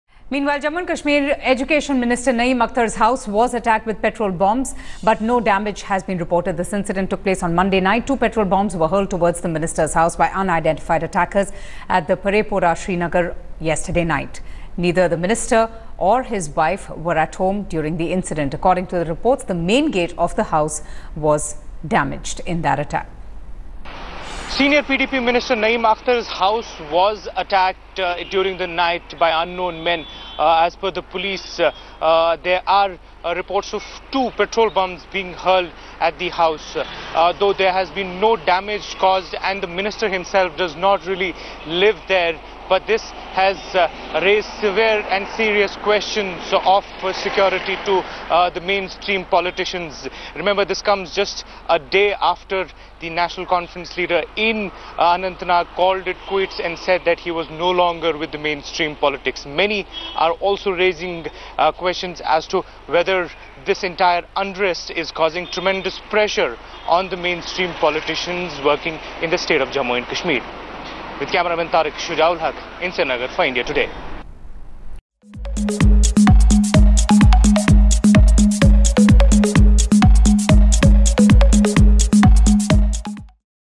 Petrol Bombs Hurled At J&K Minister, Naeem Akhtar's House In Srinagar
Jammu & Kashmir Education Minister, Naeem Akhtar's house was attacked with petrol bombs, but no damages has been reported. Two petrol bombs were hurled at the Minister's house by unidentified attackers at Paray Pora, Srinagar.